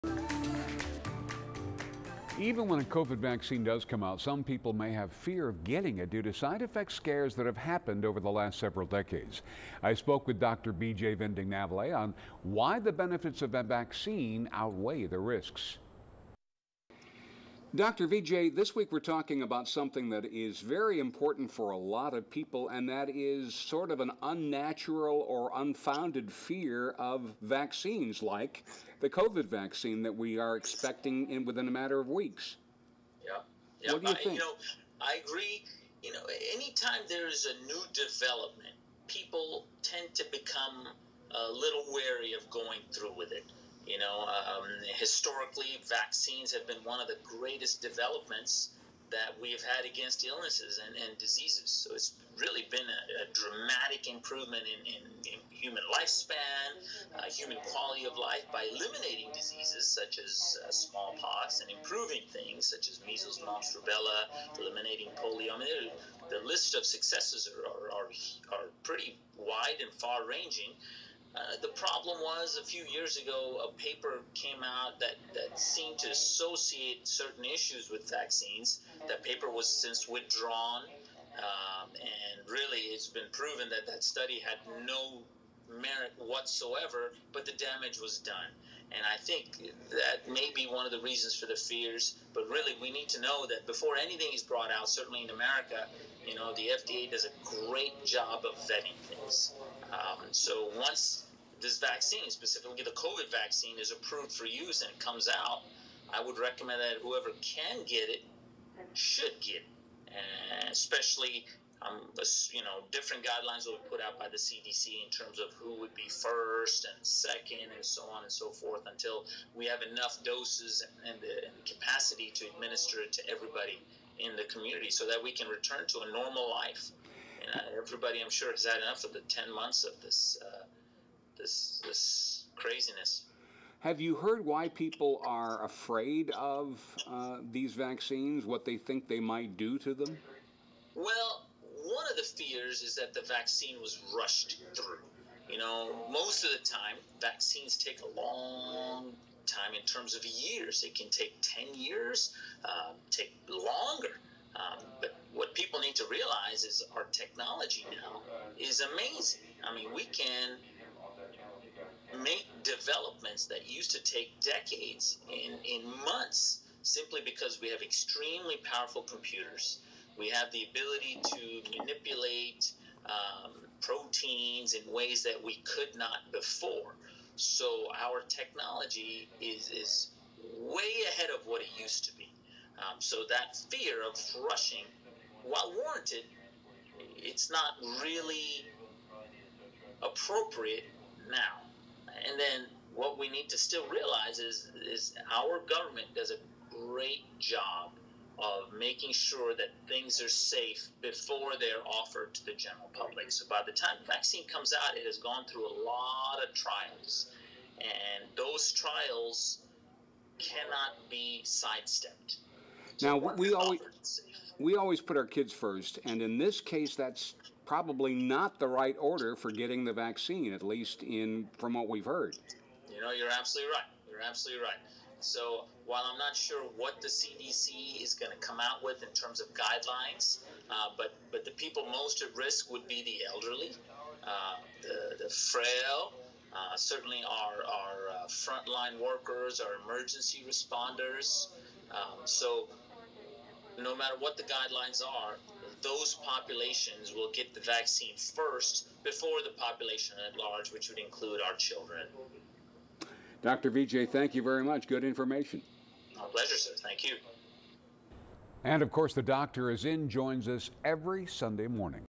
Dr. Is In - The Fear of COVID-19 vaccine
We spoke with Dr. Vijay Bindingnavele on why the benefits of a vaccine outweigh the risks.